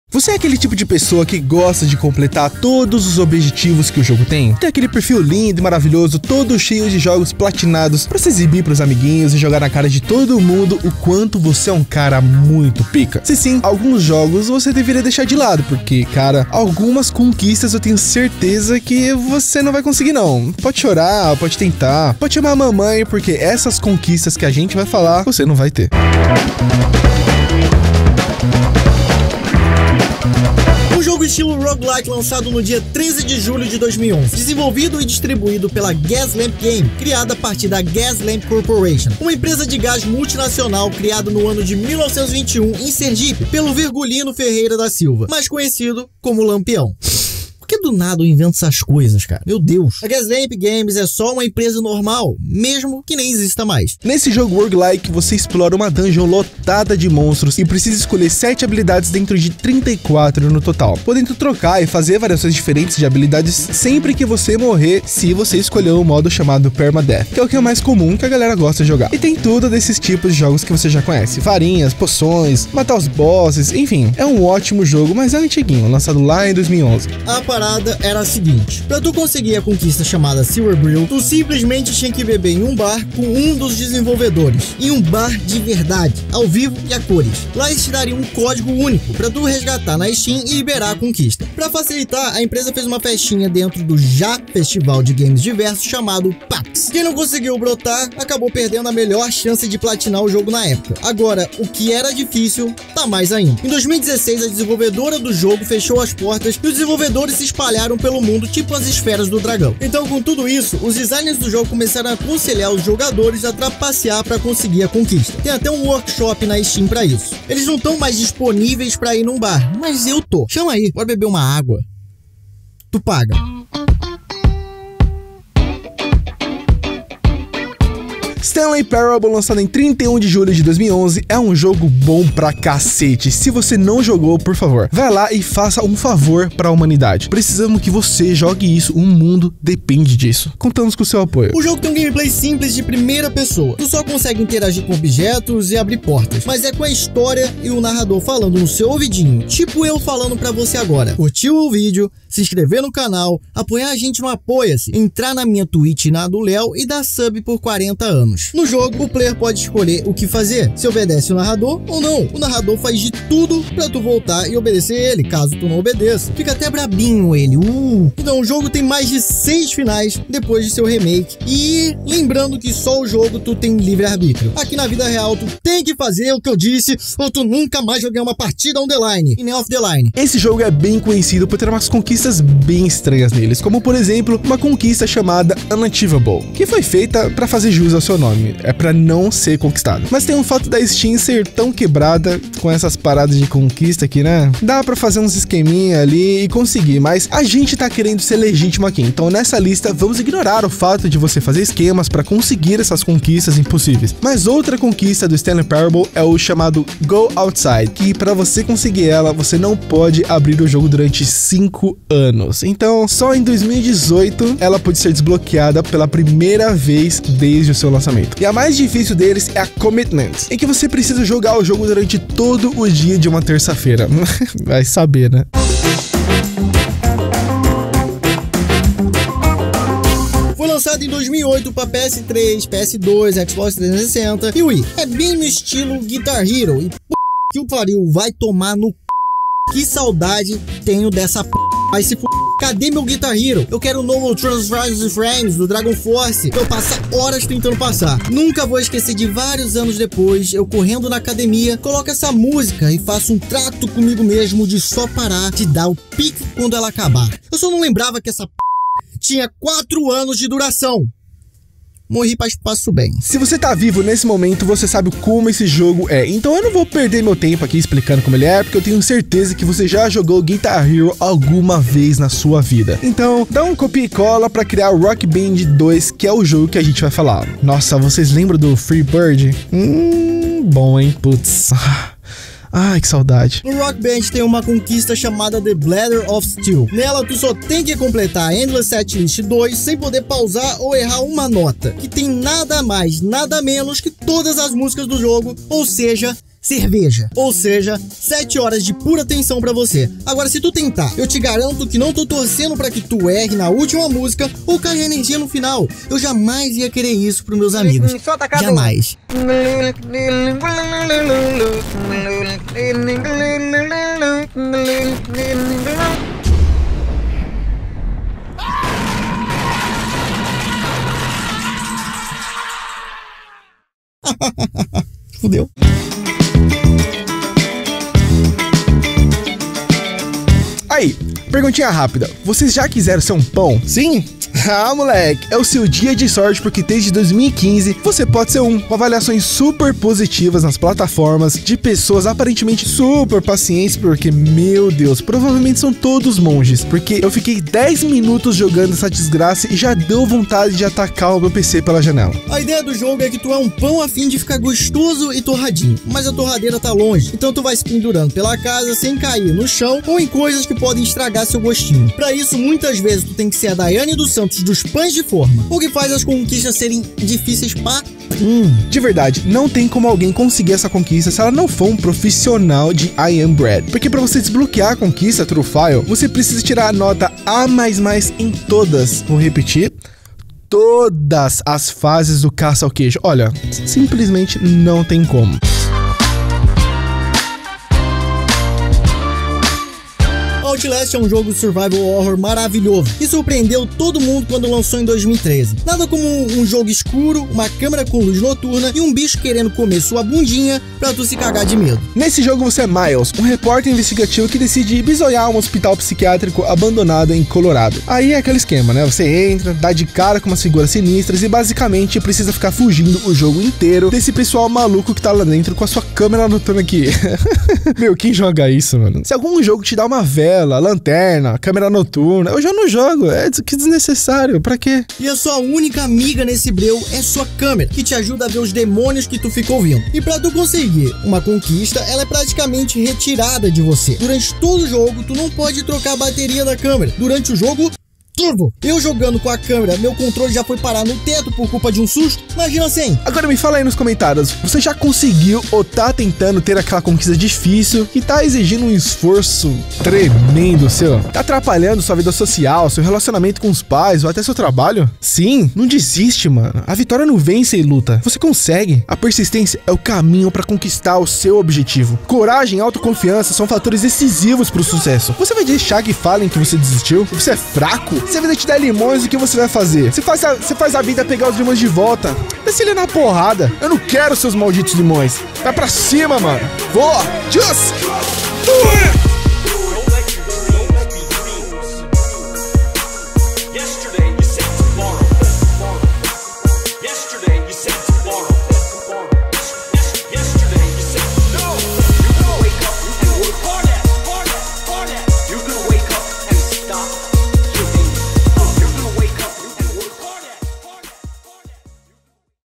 Troféus e Conquistas ABSURDAMENTE Difíceis
Você já viu quais são os objetivos mais difíceis dos games pra você conseguir sua platina de qualidade? Se liga aí, tenta não chorar...

ROTEIRO E NARRAÇÃO:
Leo